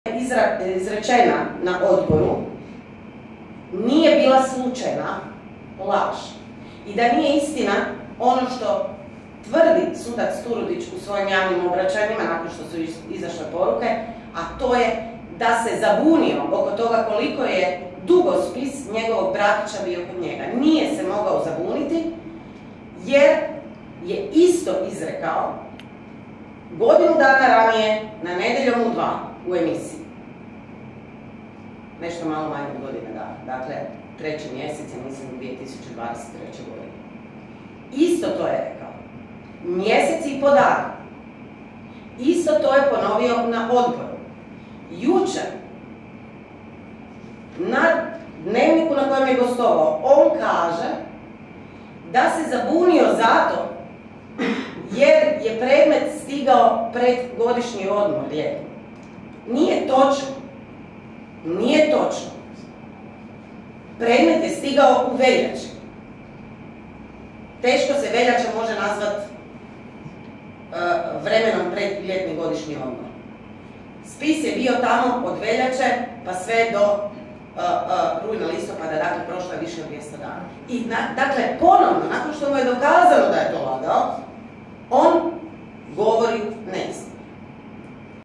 Benčić: "Turudićeve laži nisu slučajne"
Klub zastupnika Možemo održao je  konferenciju za medije na temu Aktualna politička situacija a govorila je zastupnica Sandra Benčić.